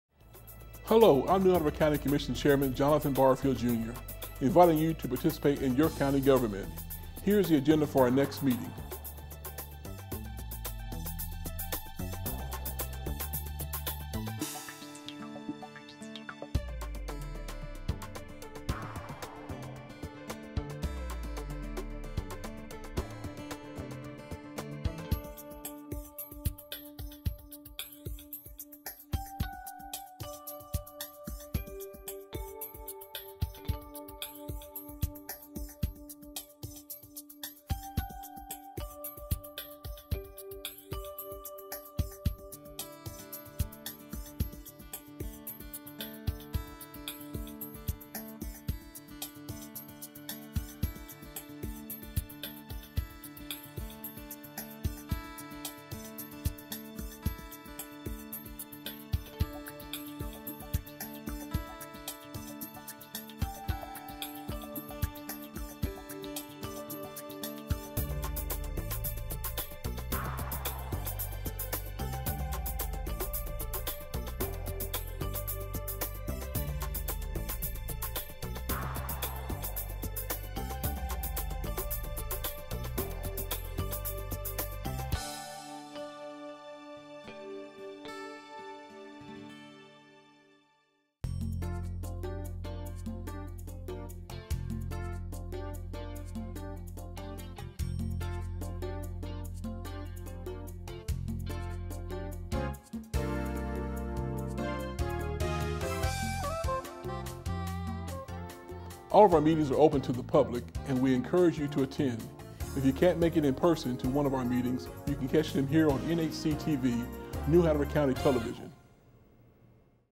Agenda Items May 2
Agenda items for the May 2nd Board of Commissioners meeting